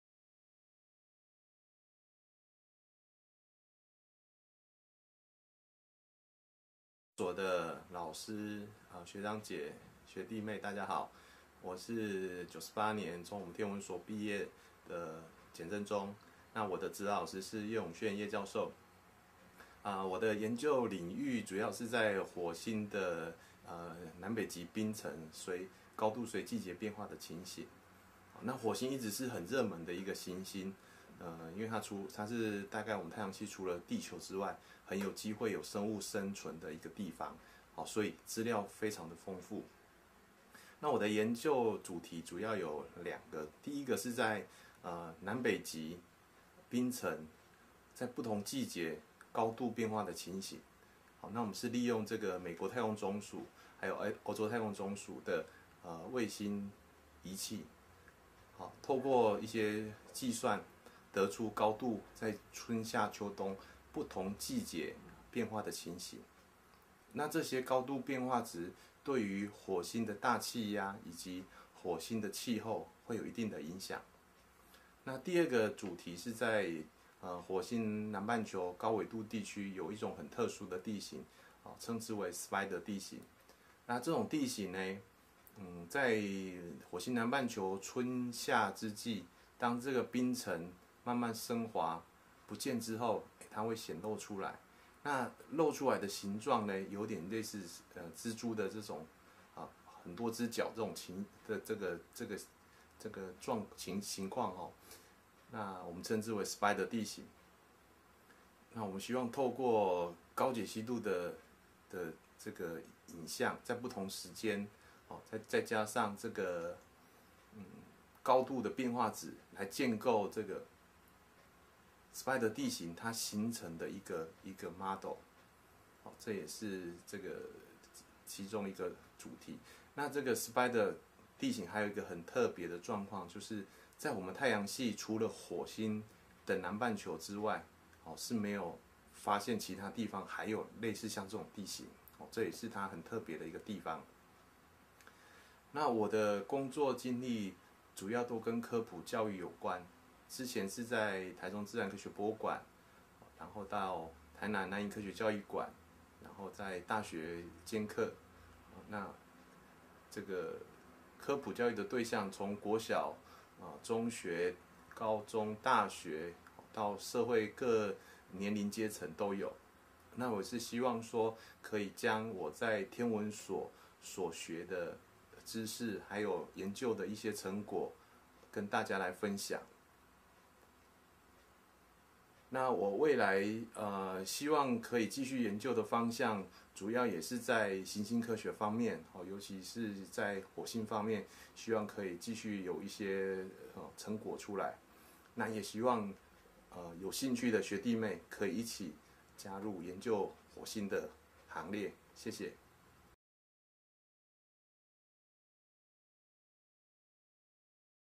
簡正忠
大家好，我是簡正忠，民國98年畢業於天文所博士班 。我的指導老師是葉永烜教授，研究領域主要是在火星南北極冰冠的季節性變化，透過美國太空總署及歐洲太空總署的探測資料進行分析，由於火星是太陽系中除了地球之外很有可能有生物存在的星球，各式各樣的探測儀器傳回許多最新資料，因此對行星科學有興趣的學弟妹，火星研究是很好的選擇之一。在我的研究中主要將衛星測得的南北極冰冠高度資料經過計算後，得出這些冰冠在一年四季中高度的變化值，這些高度變化值可以了解在季節變化時，有多少的乾冰昇華到大氣中影響火星的大氣壓力，進而對火星的天氣造成影響。另一部分是利用高解析度的照片分析在火星南半球高緯度區域的特殊地形構造，這種地形在春天溫度升高、冰層開始昇華時顯現出來，到夏天冰層消失時露出像蜘蛛型的地形，這種地形也只出現在火星南半球，因此透過高解析度影像資料以及冰層高度變化來建構此種地形形成的原因。

　　我的工作經歷包括在台中自然科學博物館、台南南瀛科學教育館以及大學兼課。不同的工作，不過都是以科普教育工作為主，尤其是在天文科普教育，從國小的小朋友、高中生、大學生到社會上各年齡階層，都希望可以透過在所上學習及研究的經驗跟大家分享，正如所上老師對我們的指導一般，也期望可以藉此增加大家對天文科學的興趣。